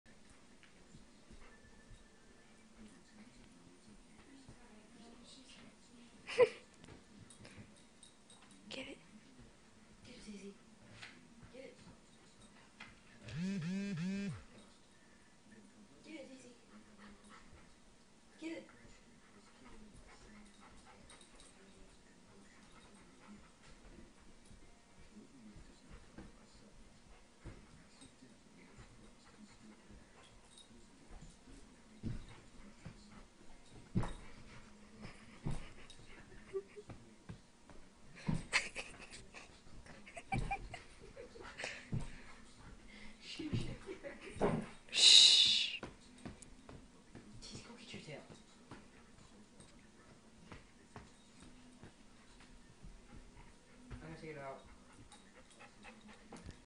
Daisy and shadows
Daisy thinks she is a cat and pounces on shadows